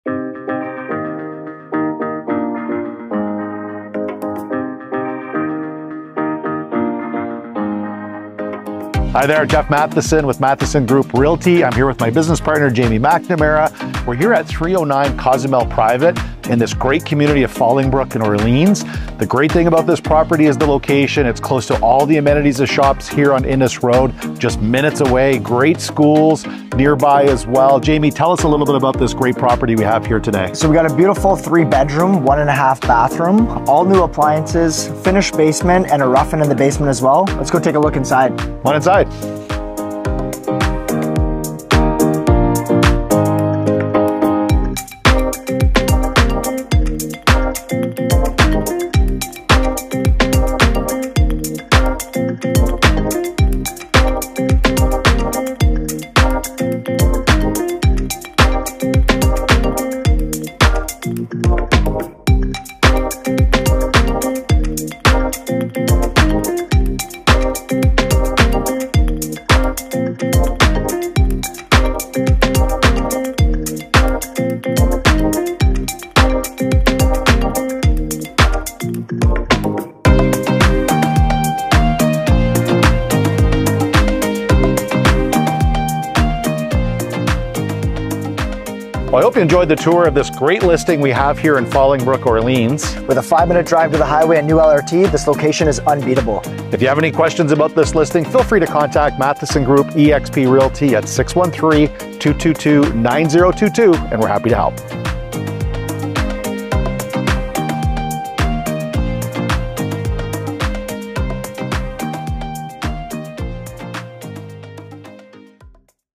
For Sale | 309 Cozumel Private | Orleans, Ontario
Welcome to this beautiful 3-bedroom, 1.5-bathroom Claridge townhome tucked away on a peaceful, secluded street in Orleans. This delightful property features a well-appointed walk-through kitchen with new never-used appliances that opens to a private backyard with no rear neighbours. The main level is adorned with rich hardwood floors. The fully finished basement offers a flexible space, perfect for creating a cozy family room or entertainment hub, with a rough-in ready for an additional full bathroom, providing potential for future expansion. Conveniently located, you’re just a short walk from essential amenities like grocery stores, gas stations, coffee shops, and the LCBO. Outdoor enthusiasts will love the nearby trails for walking, running, and biking. With easy access to the 417 highway, and a 5-minute drive to the new Phase 1 LRT Station Trim Station. Schedule your showing today! Offers presented at 6pm on Sept 9, however, the seller reserves the right to accept pre-emptive offers